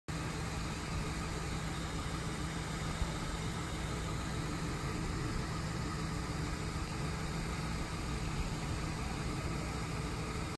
MTPD Old Car K9-7 and Middletown EMS 31-8-58 on scene
Pediatric Seizures
Additional information old K9-7 is used for road jobs but it was in the area of the call and held it down for EMS and the other officer